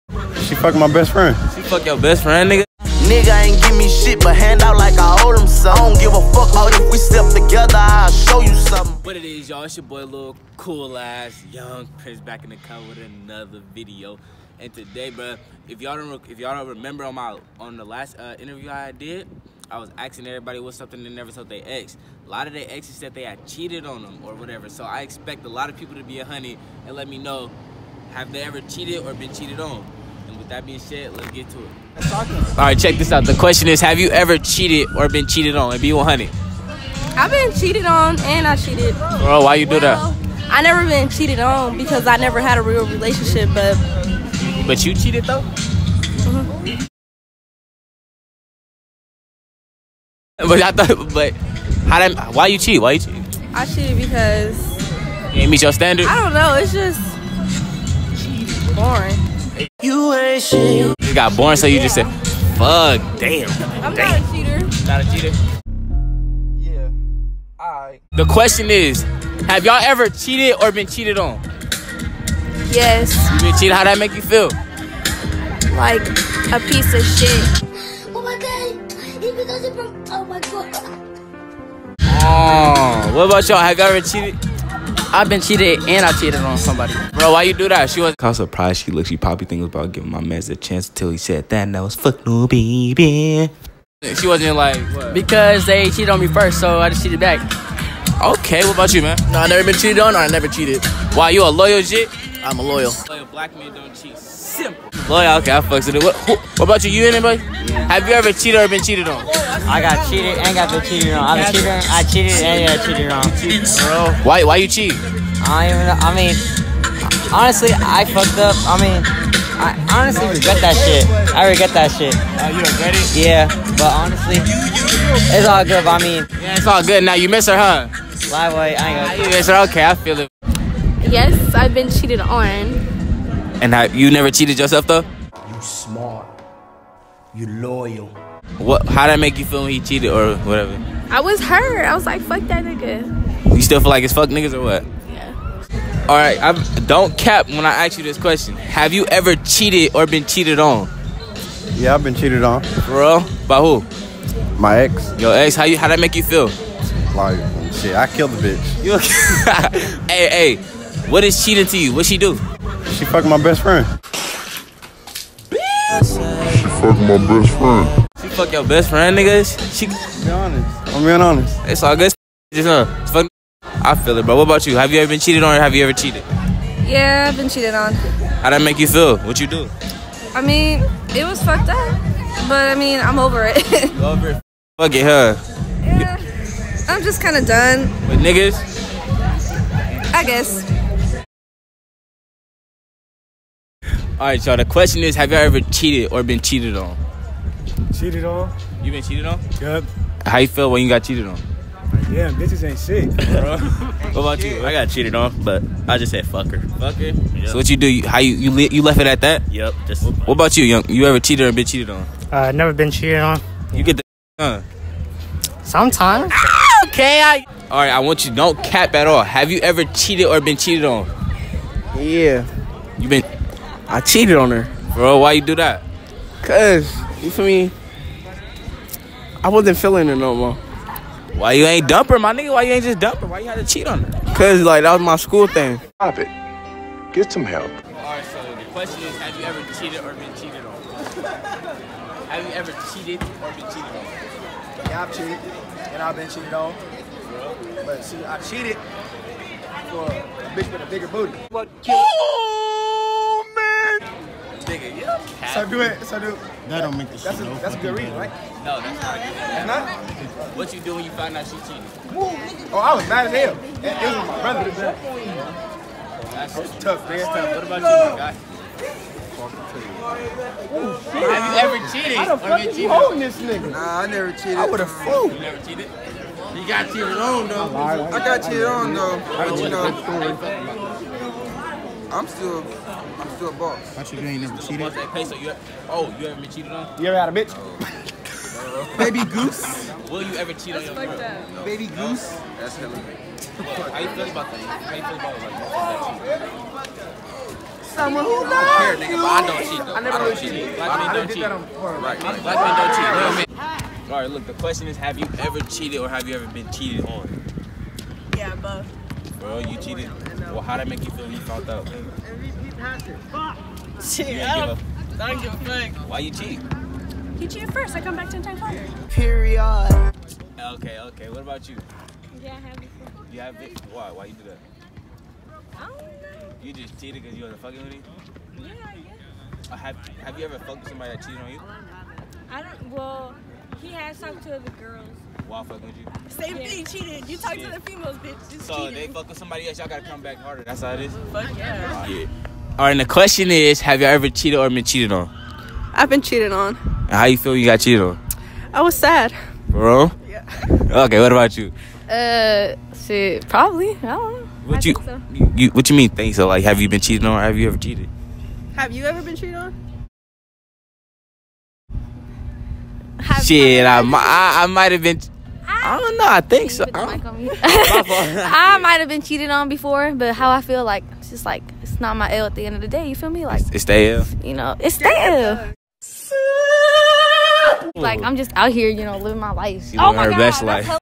Have you ever Cheated or been Cheated on | Public Interview
Have you ever cheated or been cheated on ?? Public Interview Highschool edition are there any loyal people left😂.